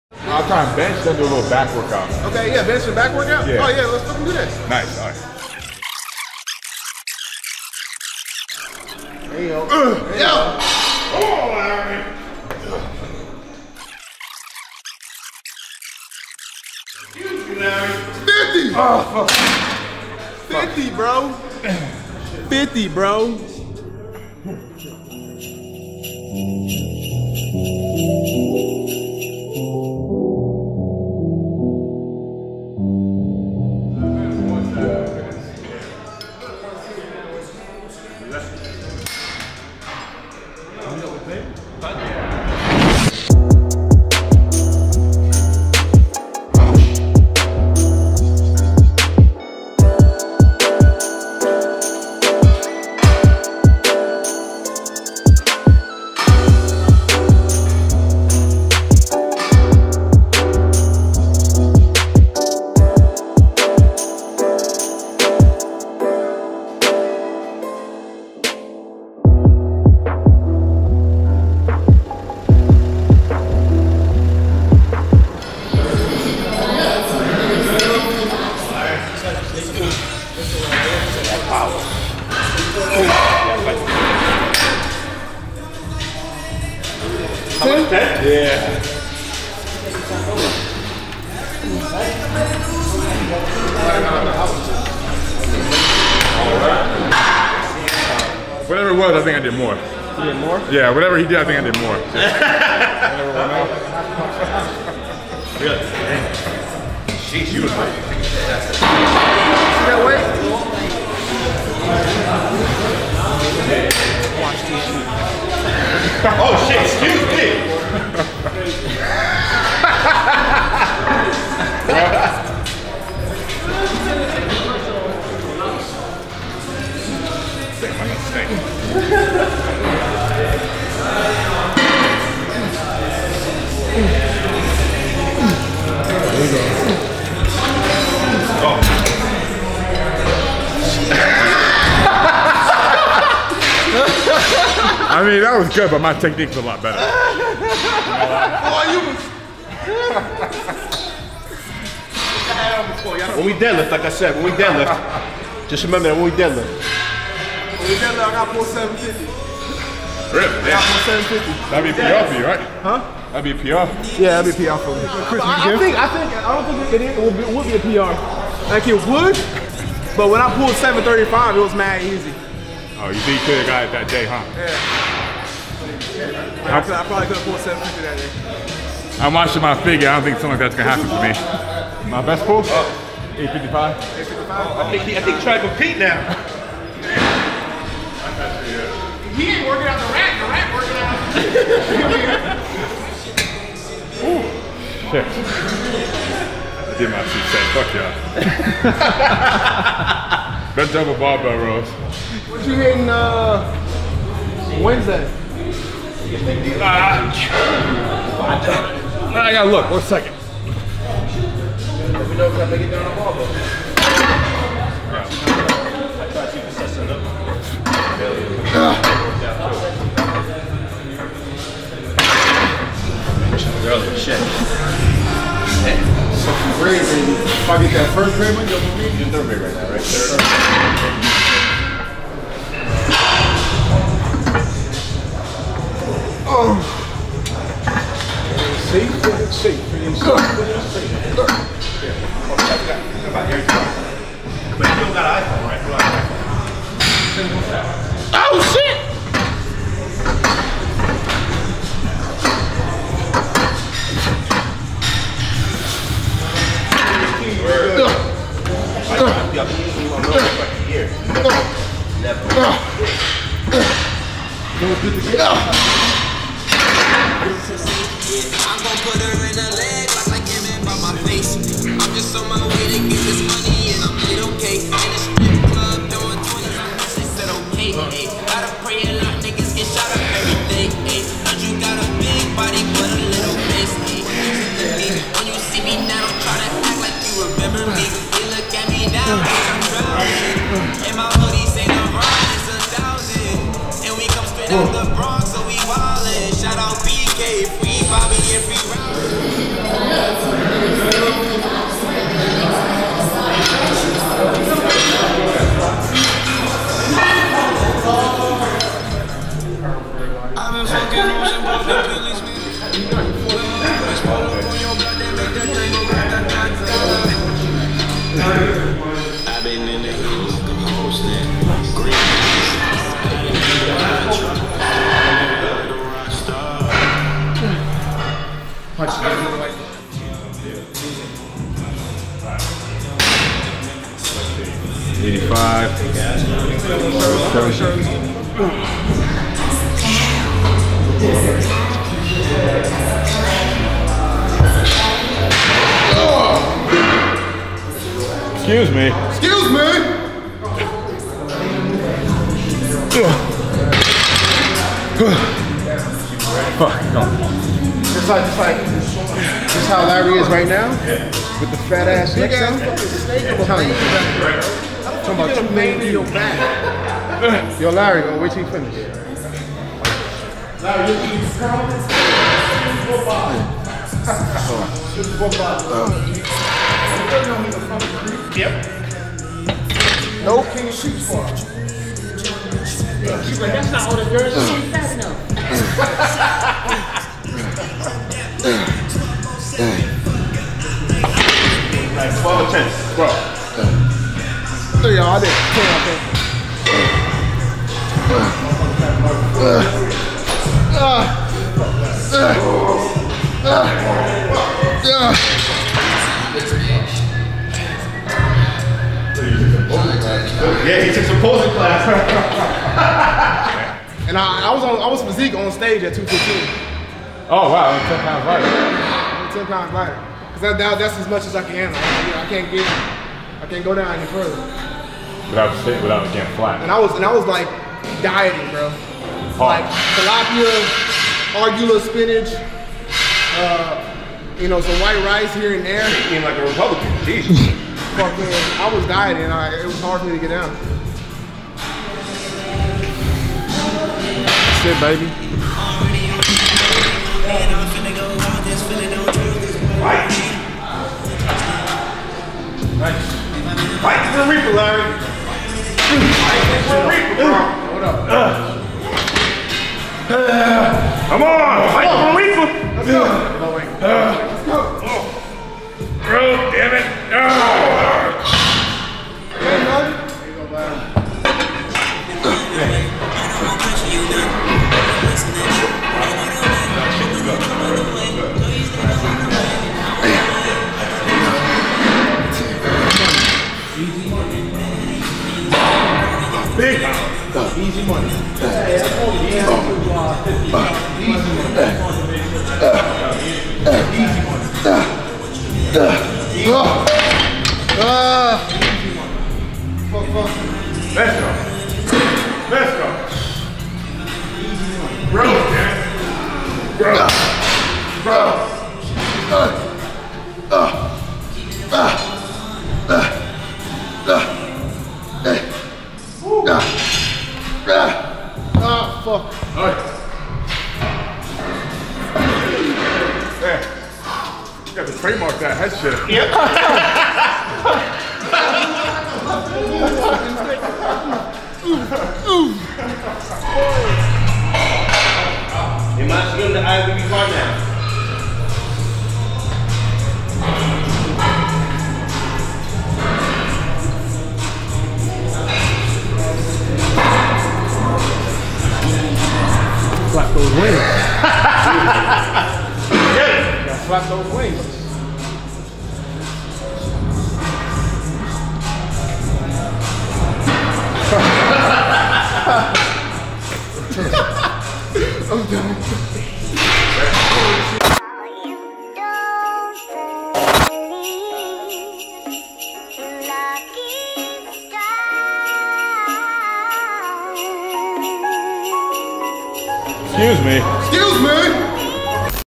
TERRON BECKHAM AND LARRY WHEELS TRAINS FOR ARMS AND BACK AND BICEPS NEW iron bars 2018
I WILL BE ADDING MORE DEDICATED FOR YOU TO GET FIT  AND BE CONFIDENT.